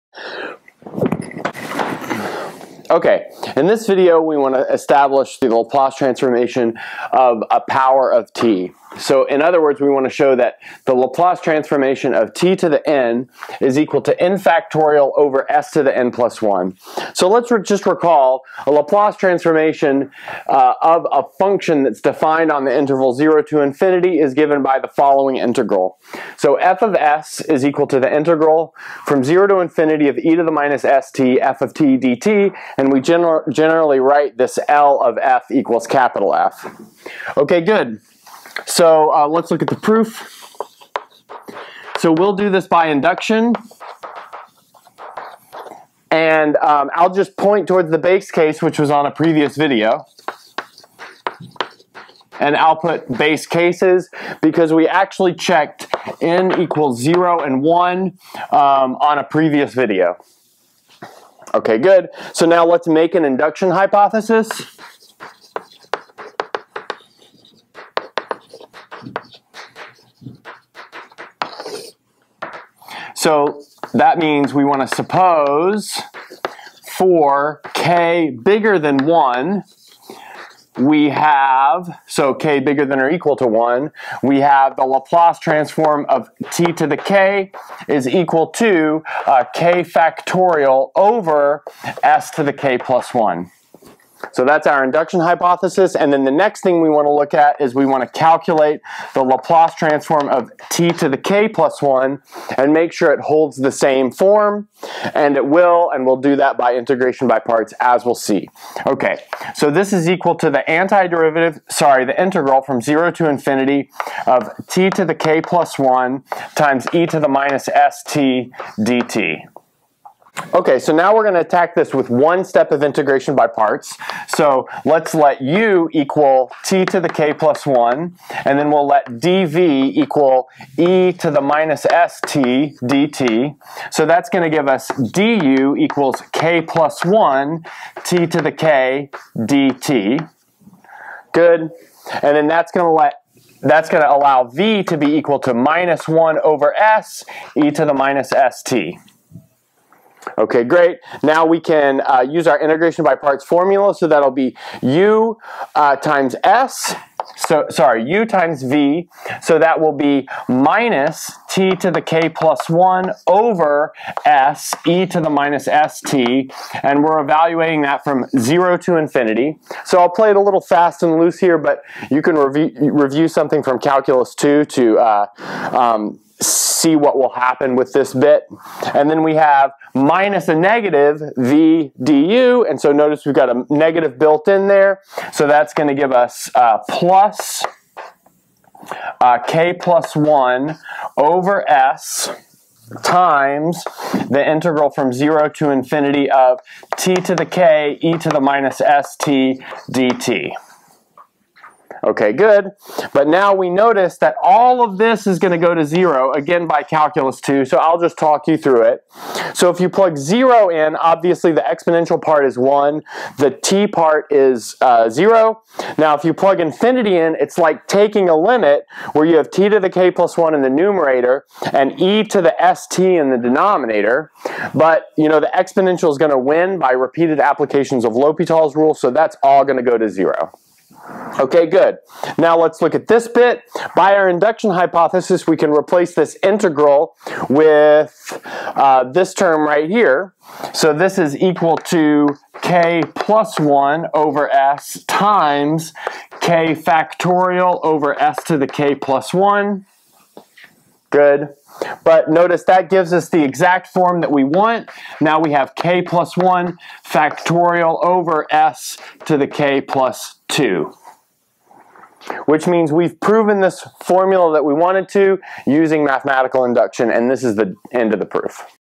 Differential Equations | What is the Laplace transform of f(t)=t^n??
On a recent trip to the moon, we establish a formula for the Laplace transform of the function f(t)=t^n.